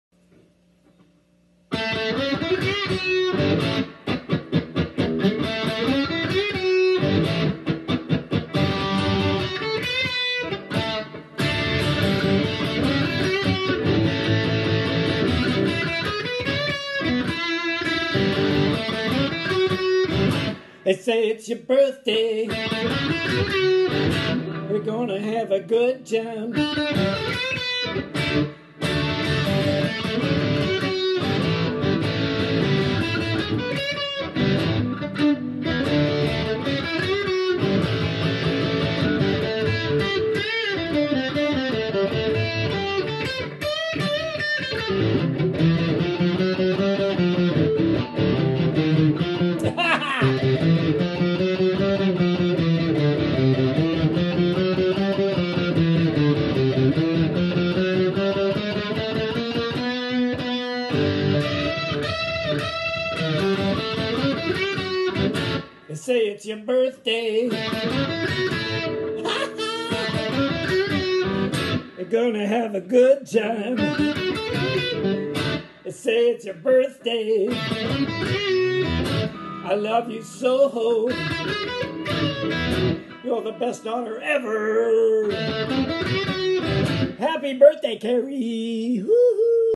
My Dad's Birthday song to me!
My Dad lives in a different country to me so this is how he wished me happy birthday! 🥰😍❤️